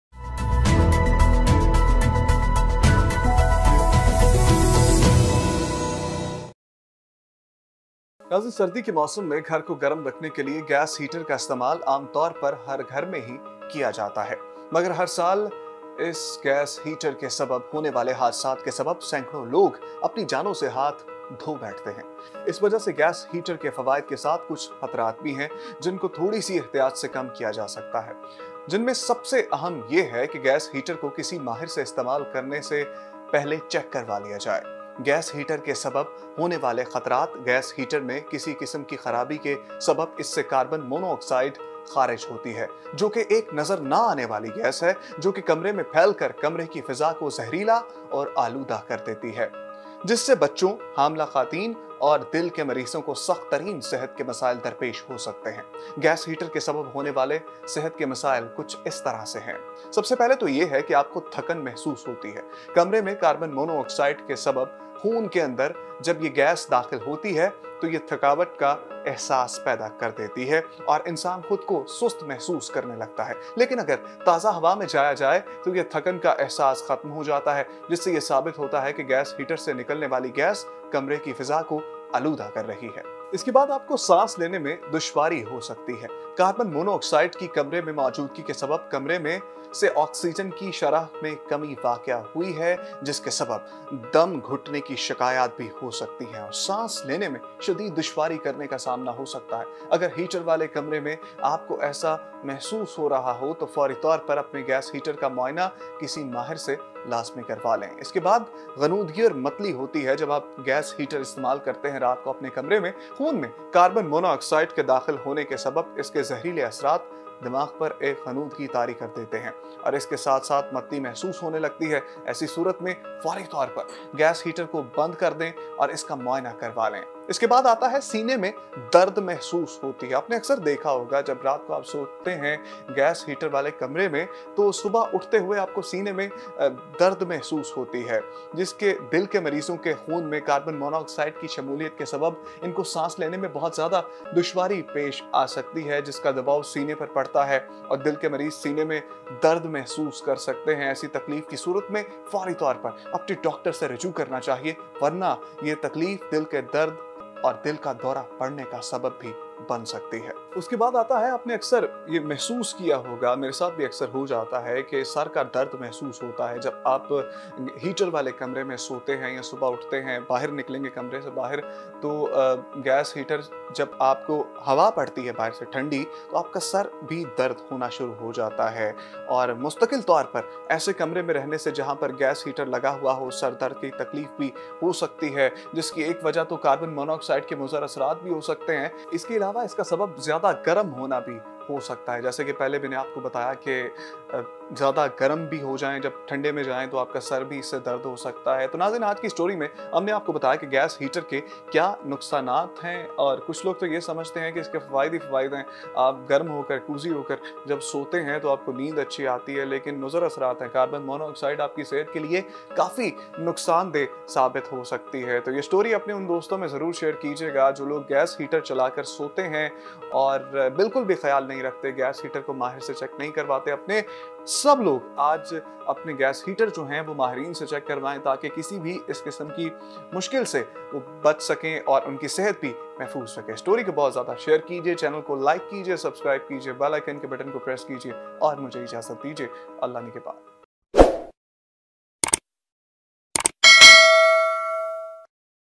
Do You Know Which Health Problems Gas Heaters Can Cause? | Talon News TV HD
A gas heater can cause severe health problems that tend to be worse or that may be caused by carbon monoxide (CO) from the gas heater only when heating is on. There is colorless and odorless carbon monoxide. It is extremely harmful to have high levels of carbon monoxide in the air, which can lead people to blackout or even die.

The risk of air pollution, including carbon monoxide, is elevated for infants, pregnant women and their unborn babies, the elderly, and people with chronic diseases such as heart and lung disease.